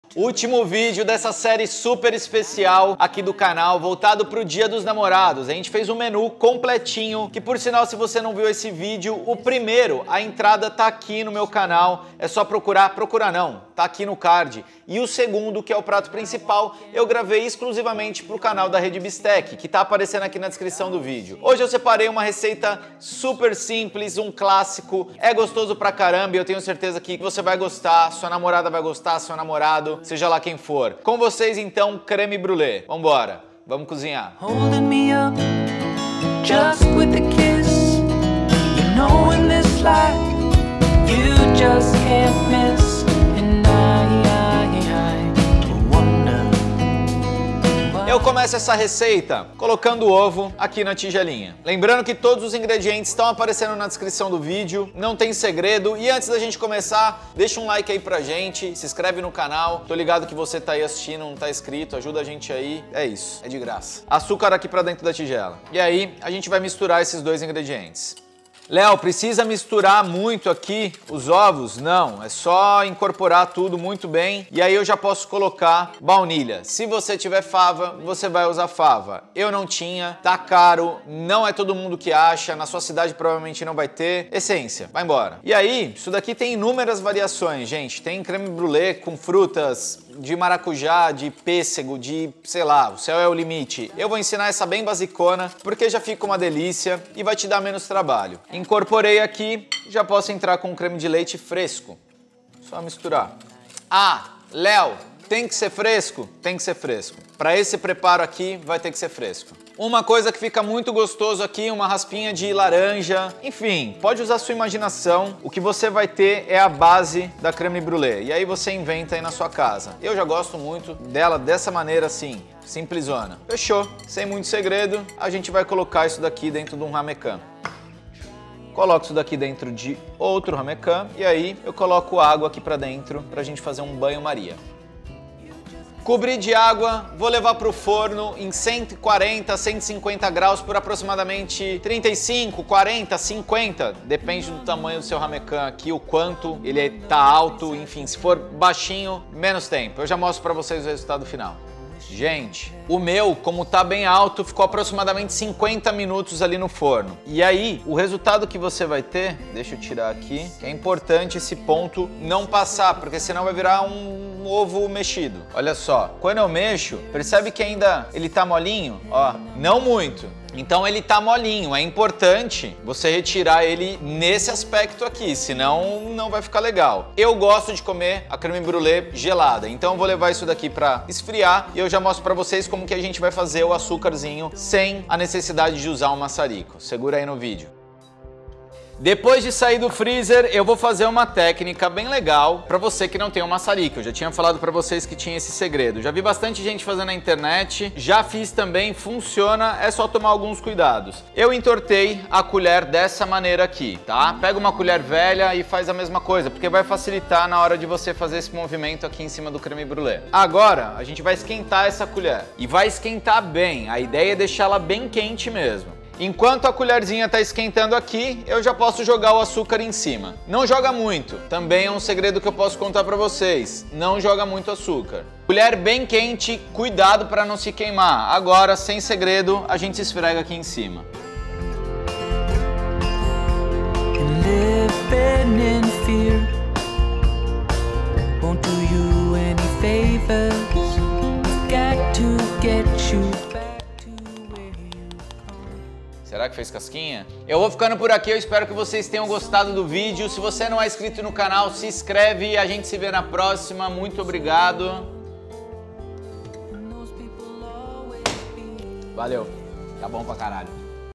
CREME BRÛLÉE 3 INGREDIENTES | Sobremesa | Jantar Romântico Parte 03
Quer saber mais? Me segue lá no instagram @leeooabreu

Ingredientes p/ uma unidade:
125ml
20gr de açúcar
2 gemas
1 colher de chá de baunilha

Produção:
- Direção de filmagem e edição:
Adriano Casanova